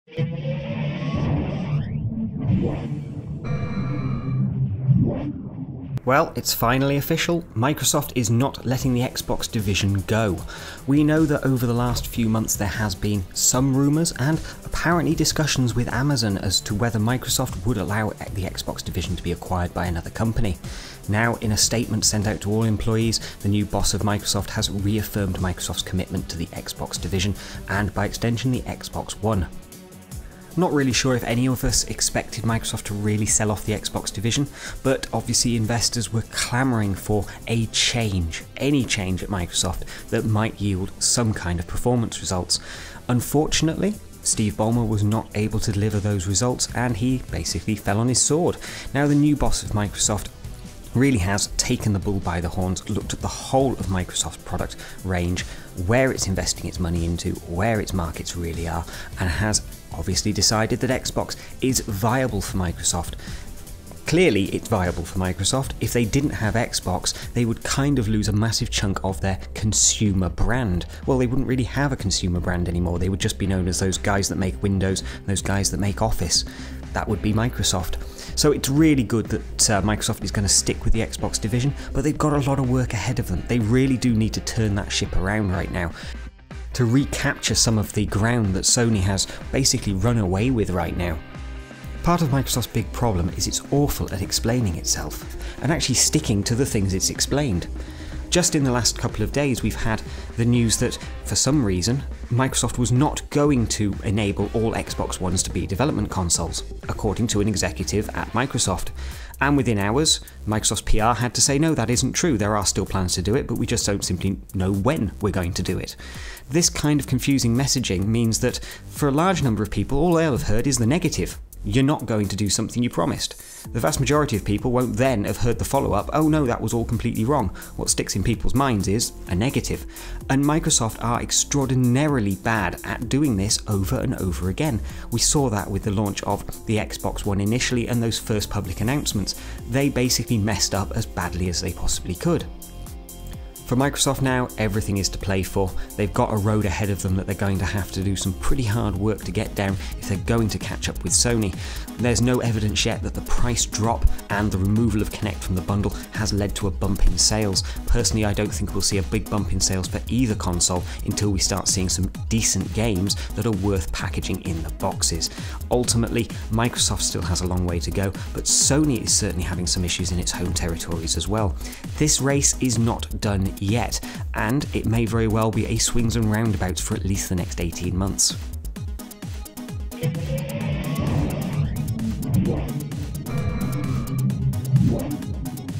Xbox Division not for sale say Microsoft - Androidizen
Xbox Division not for sale say Microsoft. It's official - Microsoft is not putting Xbox up for sale to a new owner. We didn't really expect them to but news they were talking to Amazon and others about a price at least makes it sound like they went through the motions of understanding Xbox's value to the company.

"We are fortunate to have Xbox in our family to go after this opportunity with unique and bold innovation," Nadella wrote. "Microsoft will continue to vigorously innovate and delight gamers with Xbox. Xbox is one of the most-revered consumer brands, with a growing online community and service, and a raving fan base."

The Androidizen